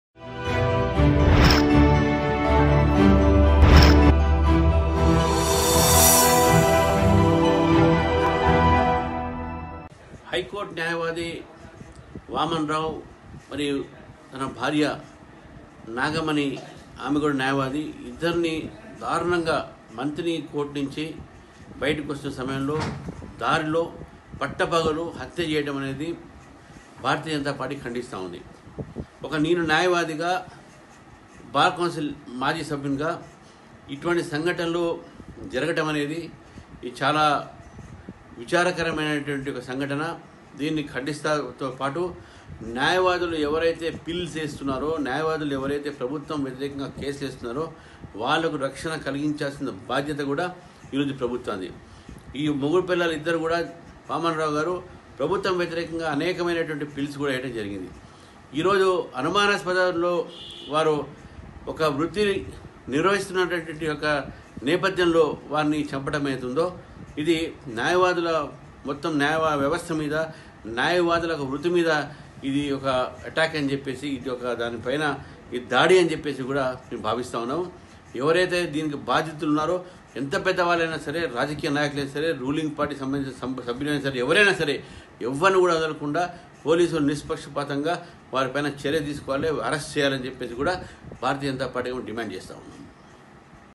వామనరావు నిందితులను వెంటనే అరెస్ట్ చేయాలి| MLC Ramachander Rao On Lawyer Vamanrao Case| ToliveluguTV
వామనరావు నిందితులను వెంటనే అరెస్ట్ చేయాలి